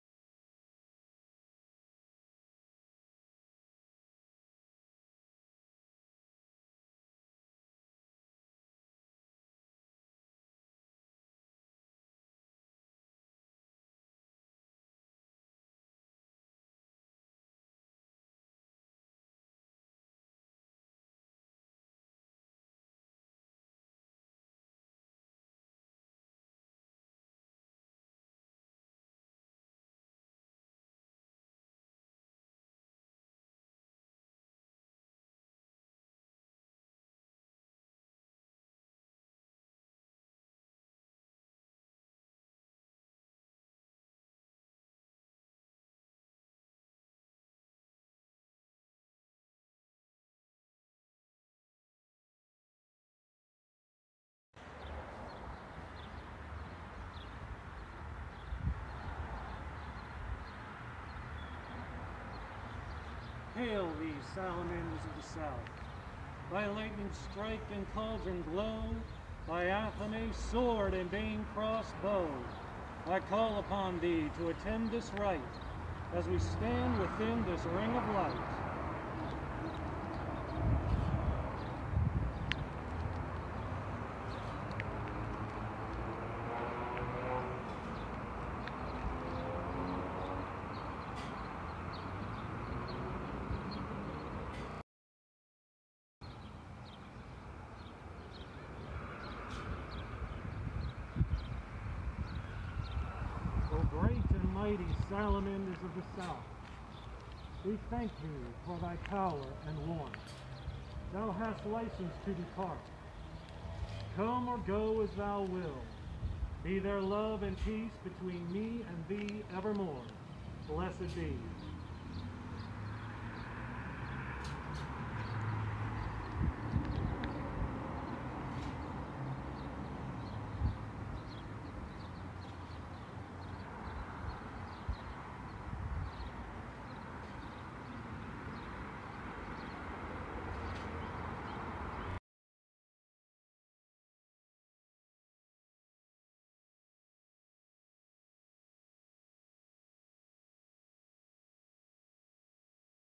04-2020 South Quarter Invocation & Dismissal - Inner Circle Sanctuary
Our High Priest performing the south quarter invocation & dismissal. We're practicing safe social distancing while handing out supplies for our lecture next week. Thought we'd treat you to a little Inner Circle Sanctuary ritual magic.